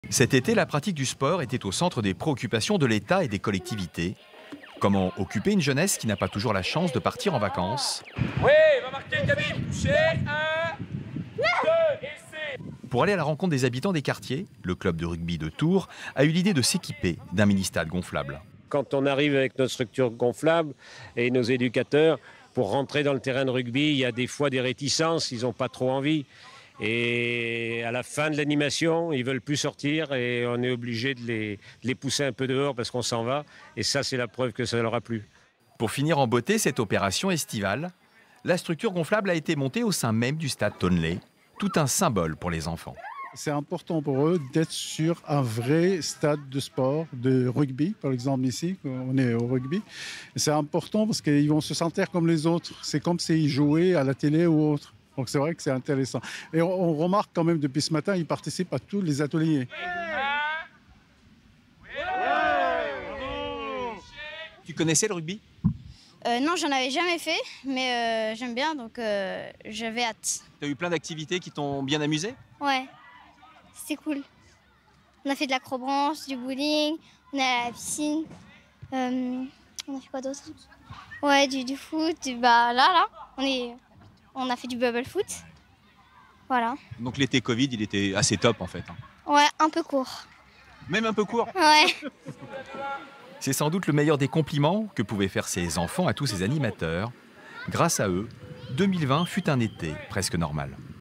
Tours : le club de l'UST de rugby sont allés à la rencontre des jeunes
Comment donner envie à des jeunes de pratiquer tel ou tel sport à la rentrée ? Tout l'été des membres de l'UST de rugby de Tours sont allés à leur rencontre.

Objectif faire naître de nouvelles vocations qui sont plus naturellement attirés par des sports comme le football ou le handball.

À Tours, Cyril Dudon, Luc Perrot